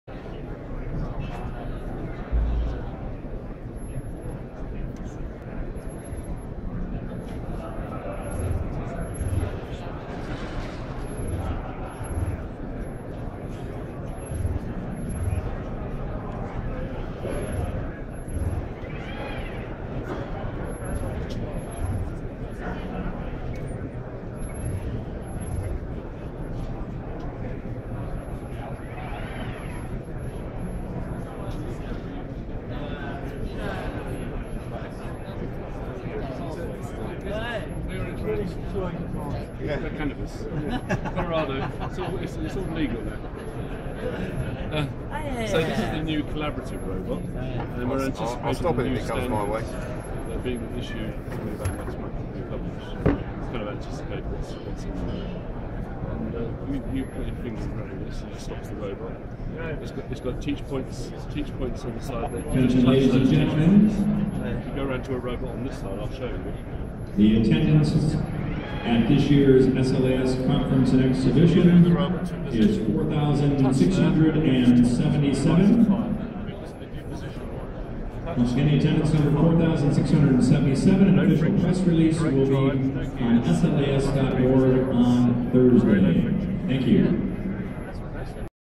PAA GX robot
Demonstrated at the SLAS laboratory automation exhibition